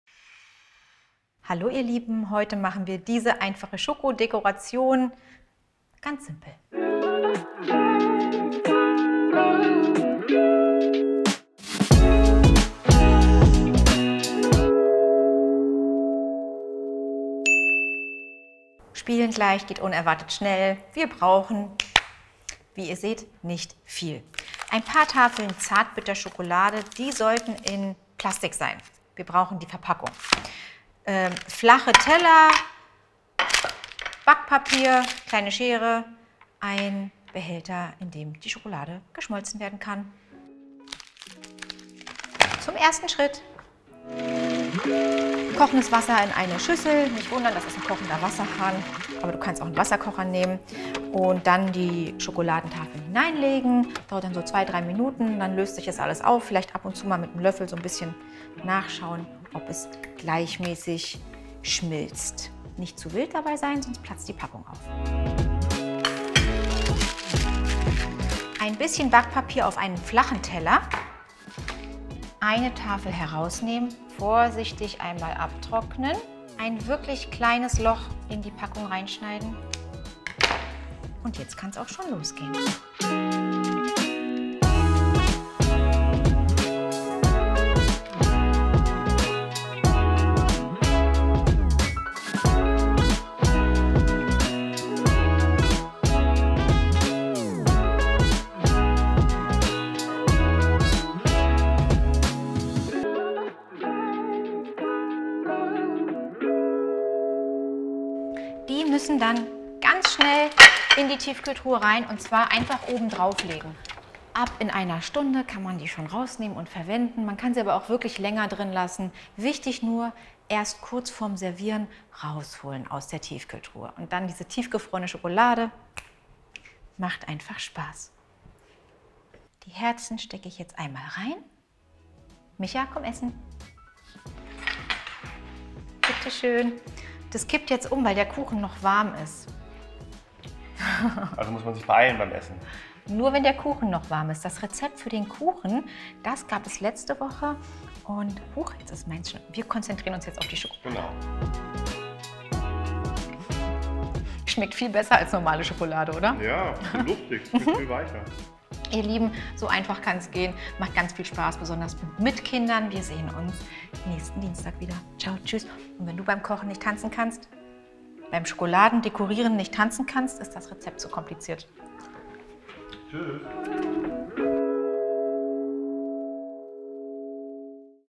Schoko-Schrift selbst machen | Trick für super einfach Deko aus Schokolade | Rezeptidee für Dessert
Man muss kein Konditor sein, um tolle Deko für zB Kuchen aus Schokolade zu machen. Ich zeige euch, wie man mit einer Tafel Schokolade ganz einfach schöne Verzierungen aus Schokolade machen kann. Kennt ihr auch so praktische Ideen, die die Arbeit in der Küche beim Kochen oder Backen vereinfachen? Dann schriebt mir eure Tricks in die Kommentare...

► Mein Dampfgarer